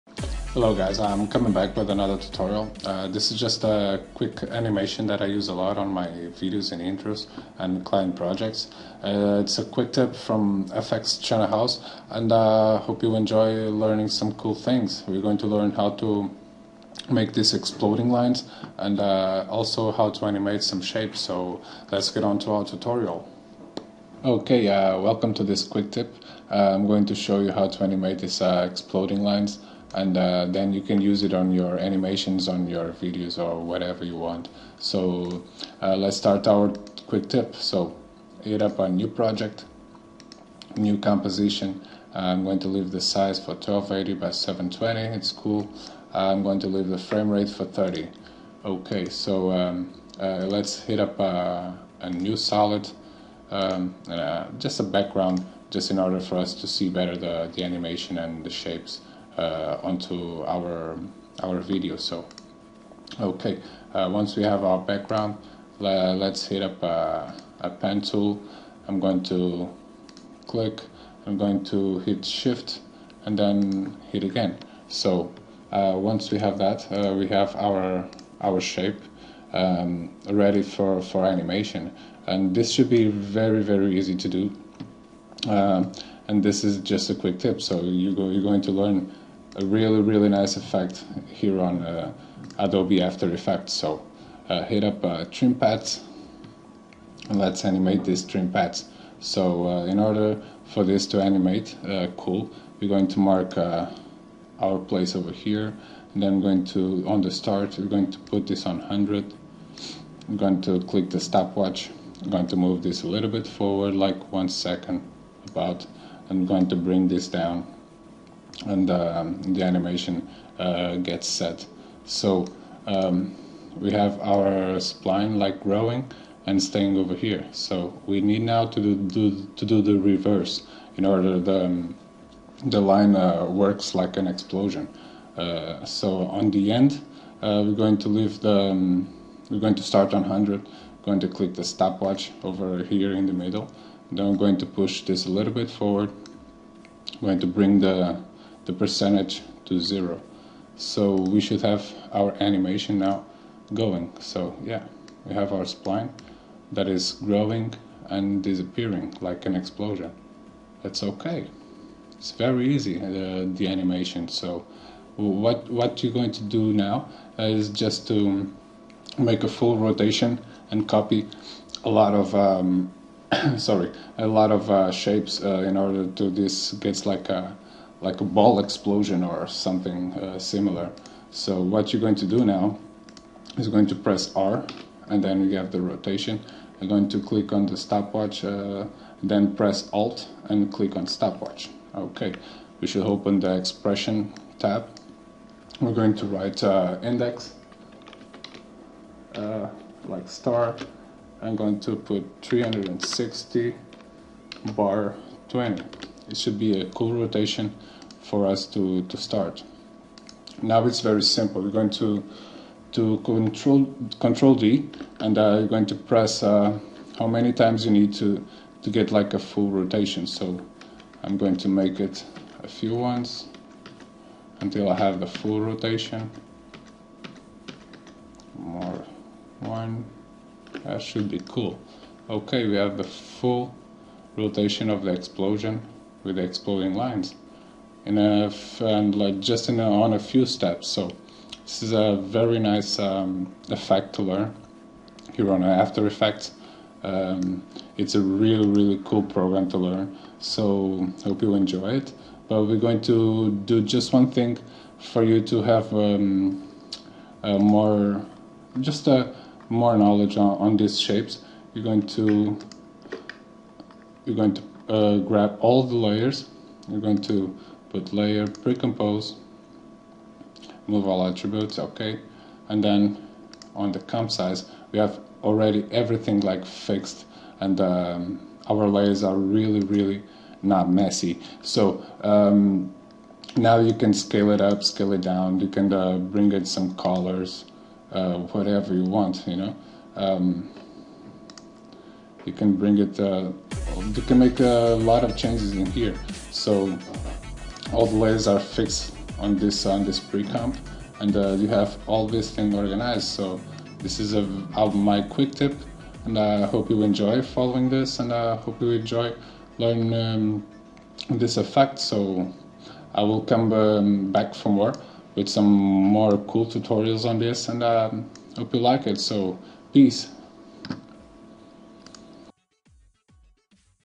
After Effects quick Tip - EXPLODING LINES
Quick tip for after effects.
Exploding lines for your motion graphics animations, quick tip from fxchannelhouse.

Enjoy, share, get connected.

Fx